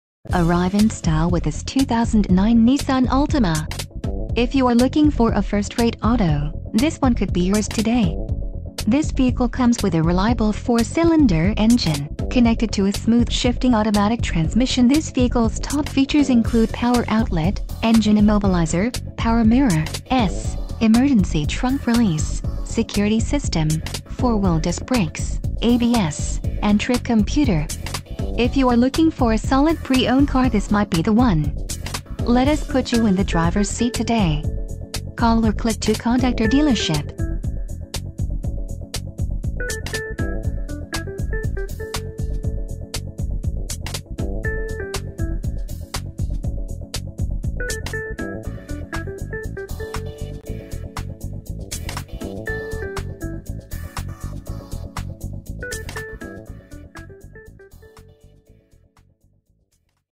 2009 Nissan Altima - TDR Auto Plaza - Kearney, MO 64060
NEW ARRIVAL!,... FRONT WHEEL DRIVE!,... HEATED SEATS!,... MOONROOF!,... WHAT A BEAUTY!,...Stunning!!! Priced below KBB Retail!!! What a value** This sweet 2.5 S is the admirable Sedan you've been thirsting for** Includes a CARFAX buyback guarantee!! Safety equipment includes: ABS, Curtain airbags, Passenger Airbag...A wealth of standard amenities means that you no longer have to sacrifice: Power locks, Power windows, CVT Transmission, Air conditioning, Cruise control... TDR Auto Plaza 'Changing The Way Used Cars Are Sold!'